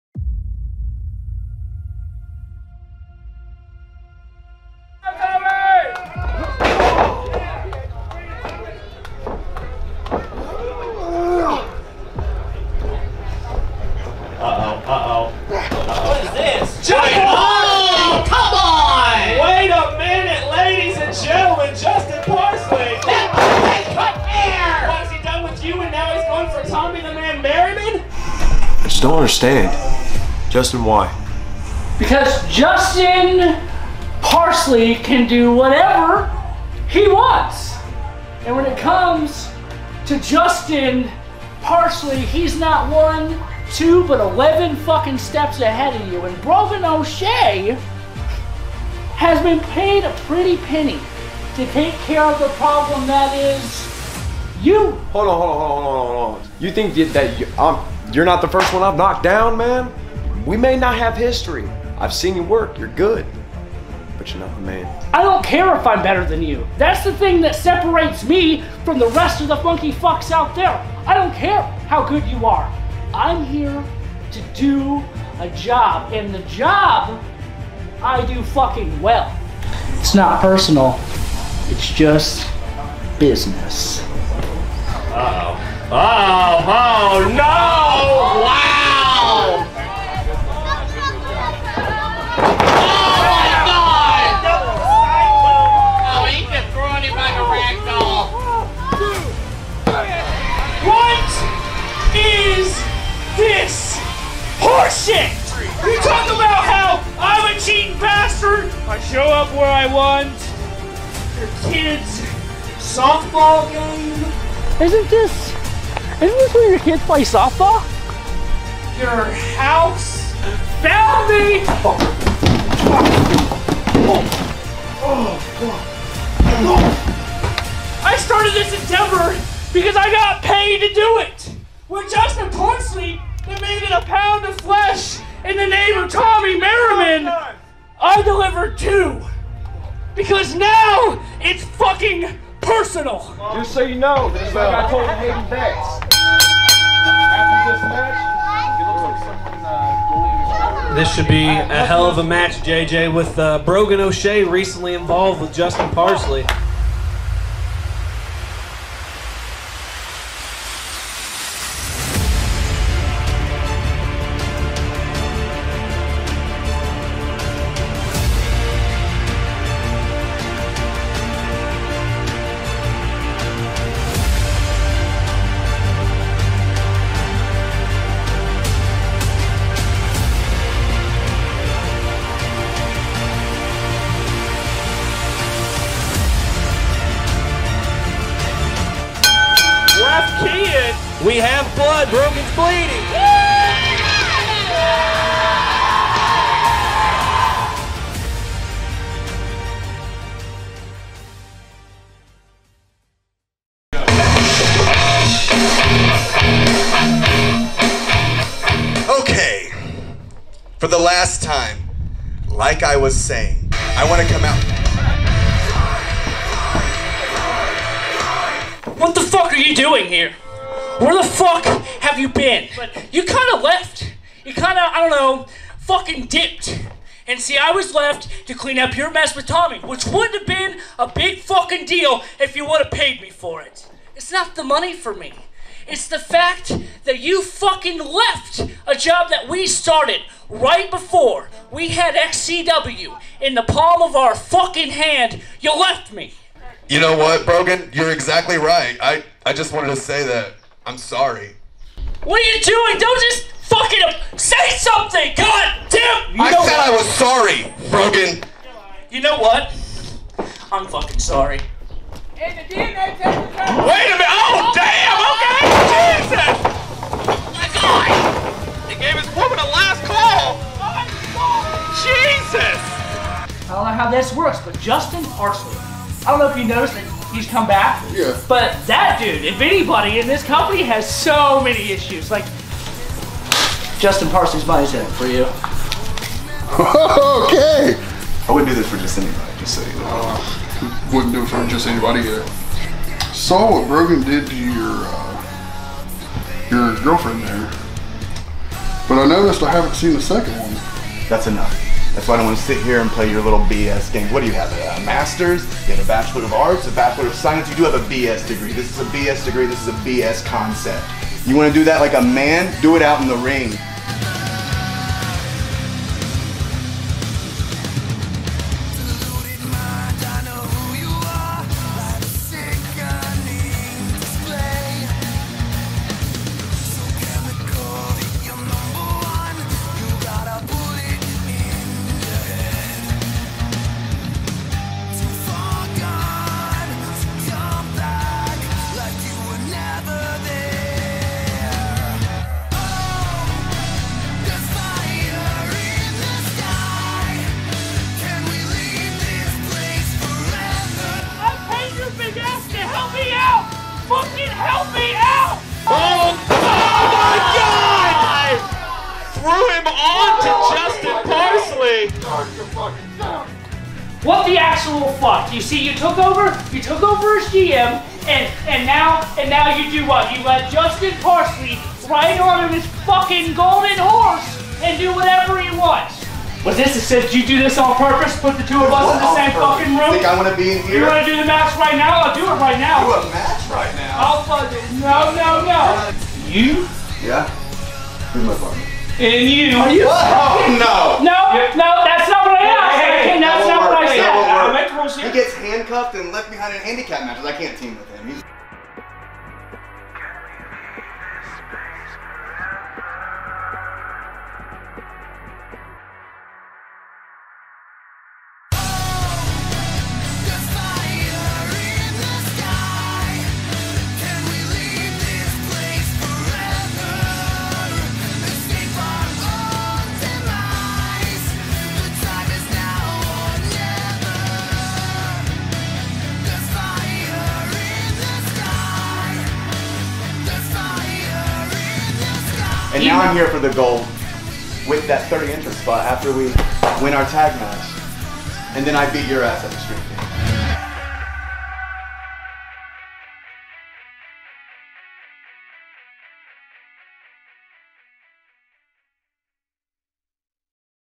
Justin Parsley vs Brogan O'Shea Package (Xtreme King 2023)
Take a look at the history between Justin Parsley and Brogan O'Shea ahead of Xtreme King 2023